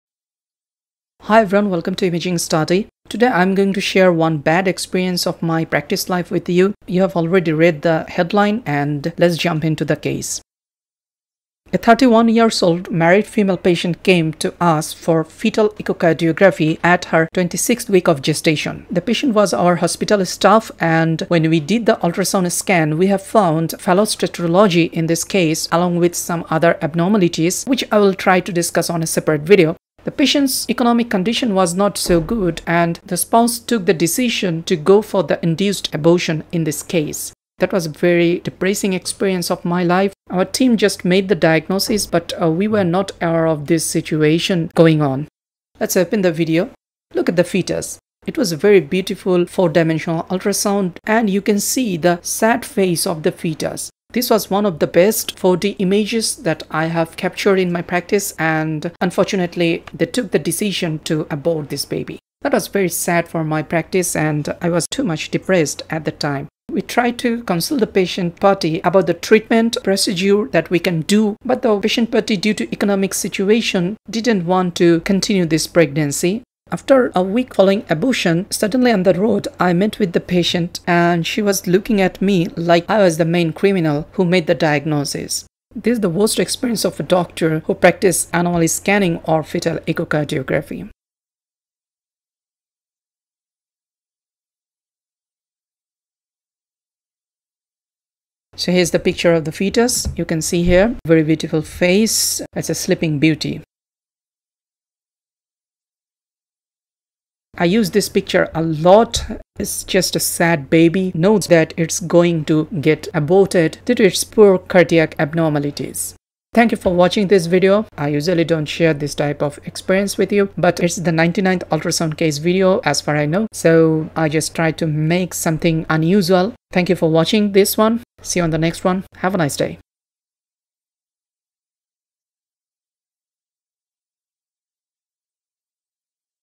4D Scan of a Fetus Before Induced Abortion Due to Congenital Anomalies || Ultrasound || Case 99
4D Scan of a Fetus Before Induced Abortion Due to Congenital Anomalies || Ultrasound || Case 99

The fetus was aborted after the diagnosis of ‘Fallot’s Tetralogy’.
Obviously, the parents had not seen these 4d pictures.
Sometimes, as a Doctor, practice is not fun at all.